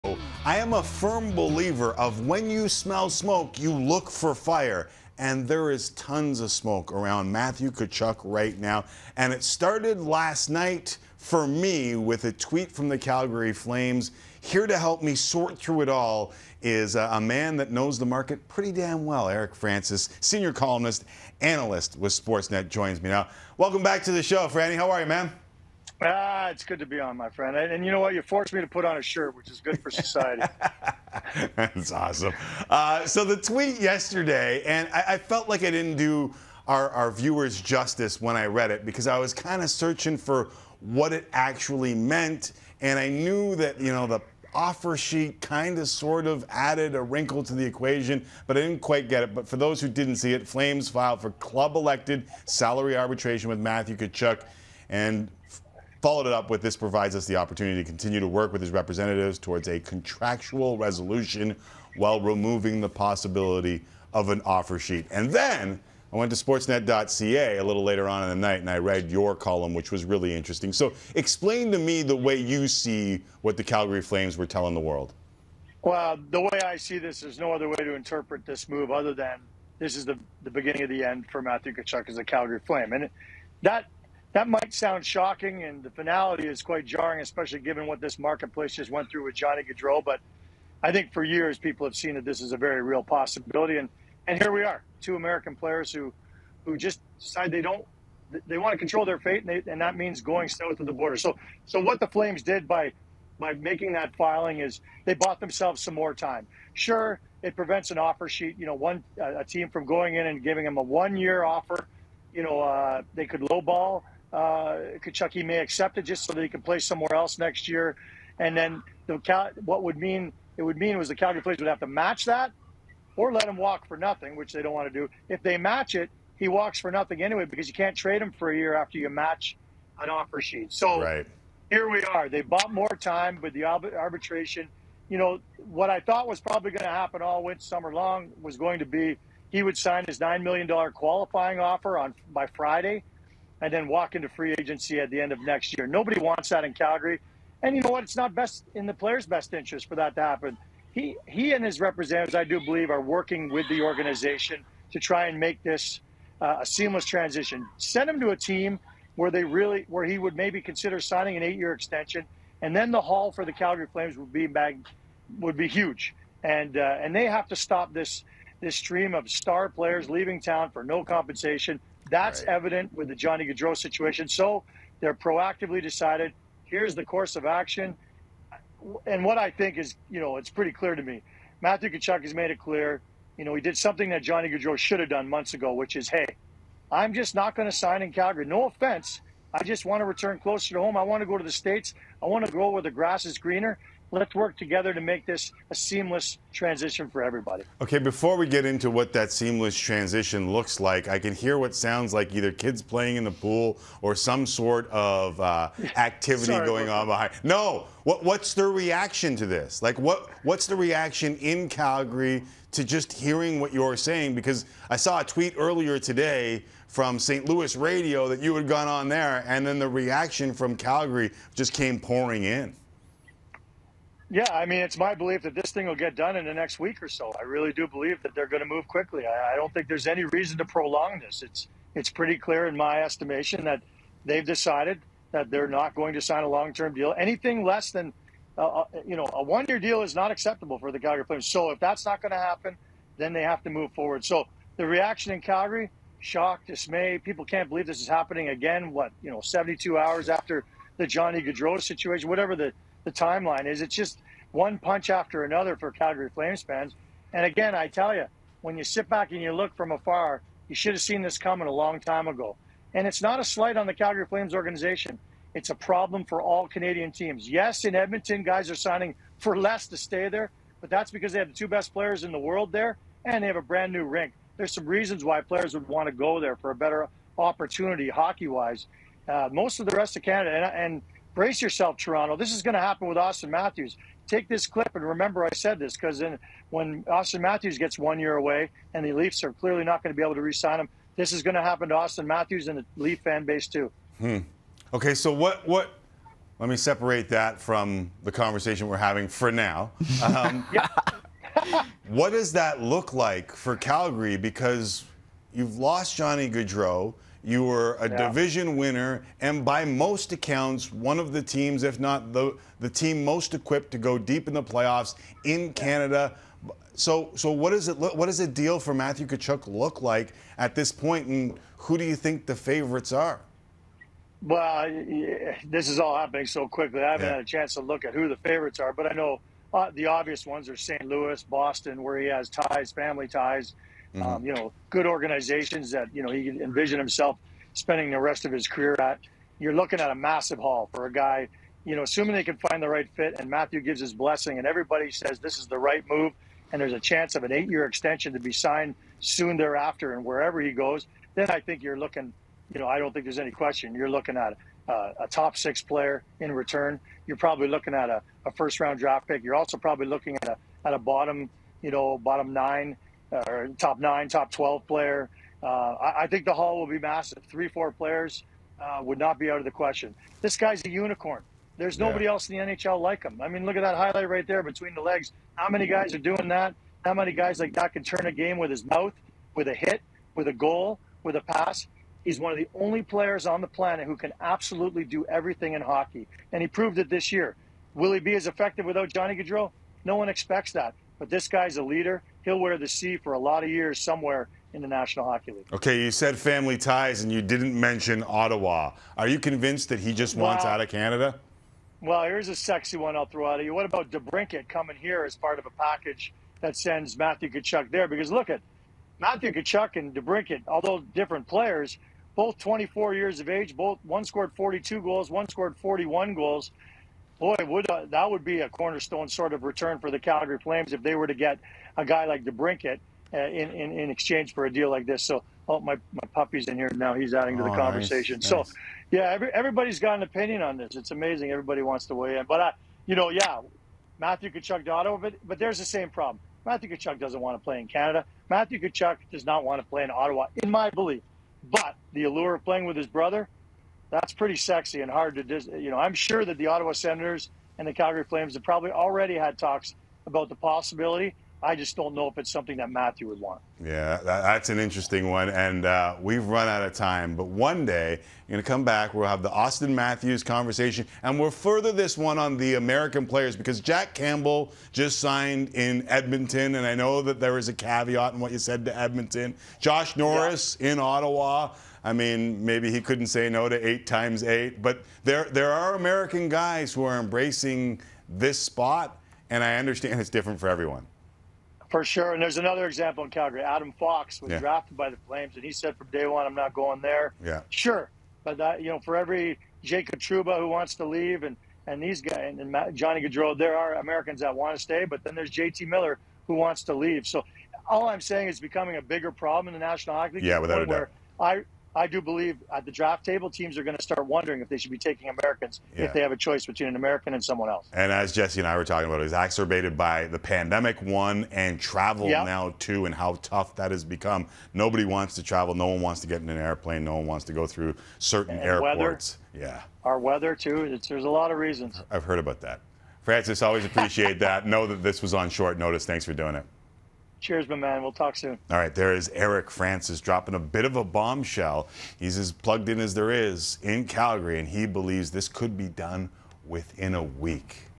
Is This The Beginning Of The End For Matthew Tkachuk As A Calgary Flame?
Eric Francis explains why Matthew Tkachuk’s time in Calgary is likely over, how contract negotiations broke down between Tkachuk and the Flames and which NHL teams could be trade suitors with Calgary.